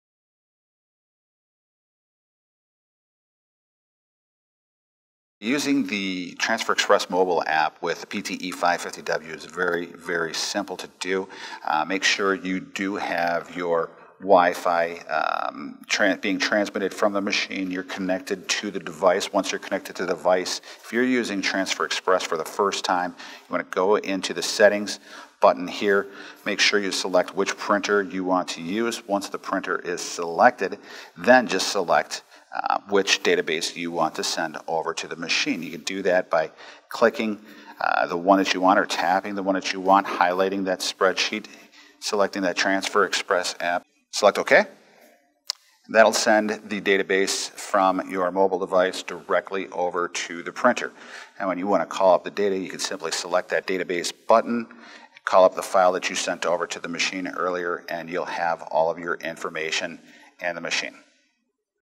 How to Use the Mobile Transfer Express App with Brother Professional Industrial Labellers
Printing from a database has never been as easy as it is with Brother’s Mobile Transfer Express app. This short video takes you through the simple steps from setting up your WiFi connection to selecting the database and then printing from your mobile device.